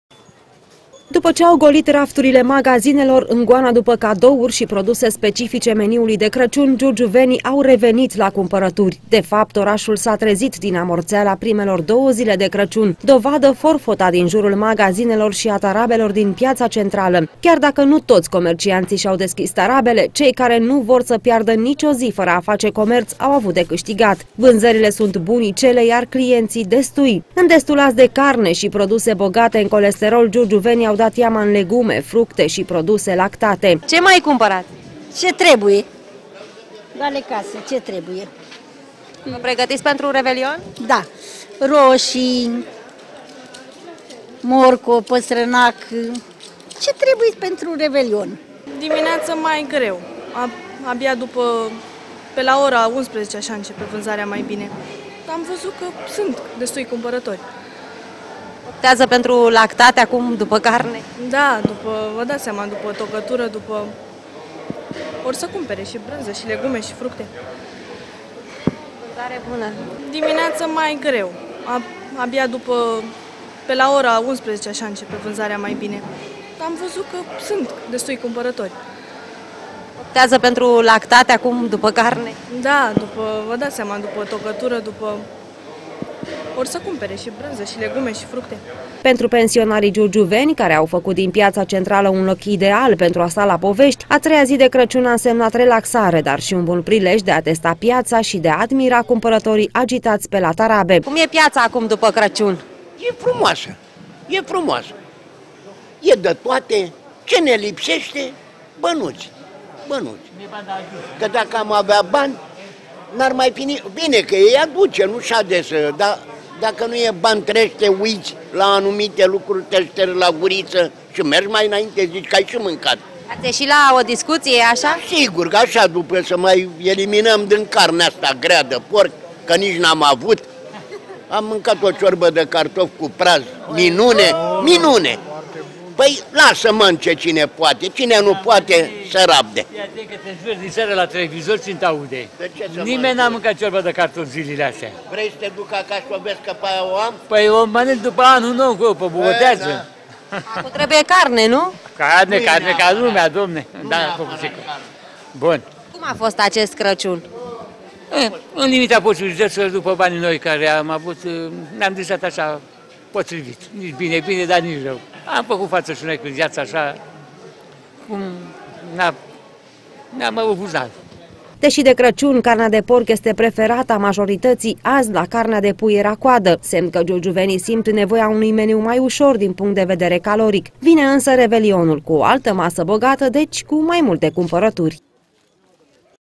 INAPOI LA CUMPARATURI
După două zile de amorțeală, Giurgiu a revenit la viață. S-au deschis magazinele, s-a deschis și piața, deci... au apărut și cumpărătorii, deloc puțini. Giurgiuvenii au pornit din nou în goana cumpărăturilor, de data aceasta pentru masa de Revelion. Totuși cele mai căutate produse au fost lactatele, fructele și legumele.